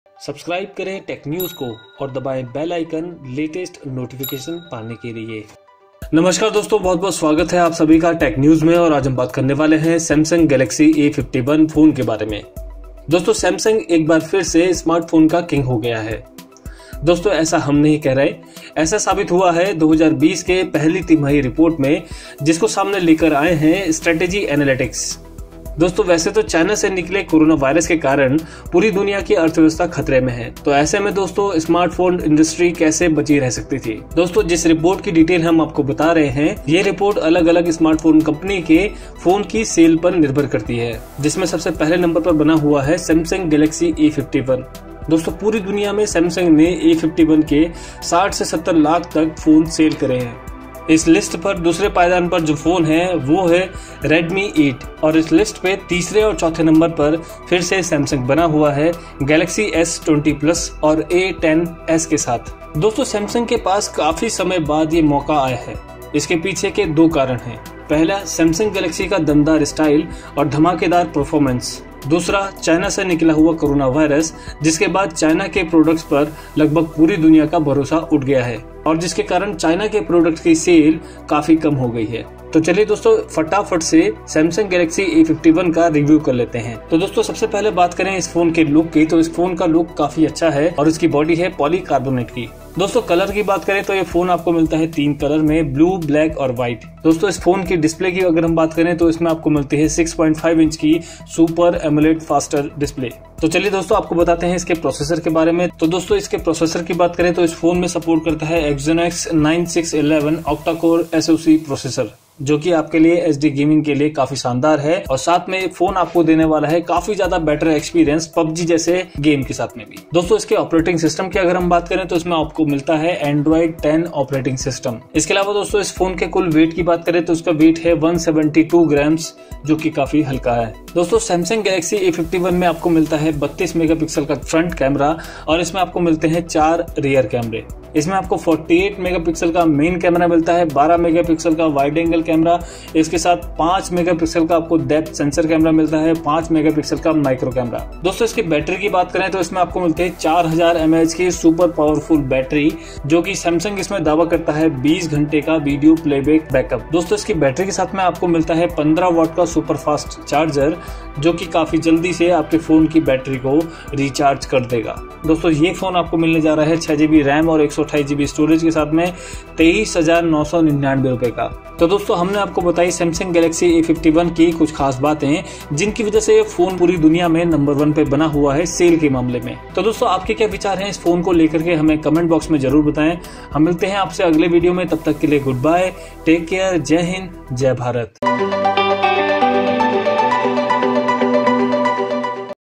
Samsung Galaxy A51 | Best Selling Smartphone 2020 | Samsung Galaxy A 51 Price in INDIA
Hello Friends Welcome in Tech News No. 51

Title: Samsung Galaxy A51 | Best Selling Smartphone 2020 | Samsung Galaxy A 51 Price in INDIA | Tech News
About This Video:
Hi Friends, In this Video we will tell you about  strategic marketing analytics.  Actually this report brings Good news for Samsung Lovers, because this report says Samsung Galaxy A51 has become the highest selling Smartphone in the word in starting months of 2020. There is two reason behind this first one is quality of Samsung Galaxy A 51 and another one reason is spread of Corona Virus From china, therefore china loose faith of many countries and these countries are not use China’s product.

There are several Qualities of Samsung Galaxy A51 but here are few of them:
Samsung Galaxy A 51  Body: Samsung provide you Polly carbonate body  in Galaxy A51, which looks Very attractive.
Colour: It comes in Three Different Colour Blue, Black and white.
Display:  You Will Get 6.50 inch Super AMOLED  Faster display
Operating System: Samsung Galaxy A51 Supports  Android 10 operating system.
Operating System: It has Exynos 9611 Octa Core SOC Processor.
Weight: Samsung Galaxy A51 is very light in wait, it has 172gm.
Front Camera: 32MP
Rear camera : 48 MP Main,  12 MP Wide angle,  5 MP Dept Sensor and 5 MP Micro.
Battery: A51 has 4000mAh Battery, Samsung is claiming  20 hours Video play backup.
Storage: 6GB RAM and 128 Storage.
Samsung Galaxy A 51 Price in India: 23,999

So Guys Watch Complete Video To Get All Facts And Updates Behind This Tech News.

Team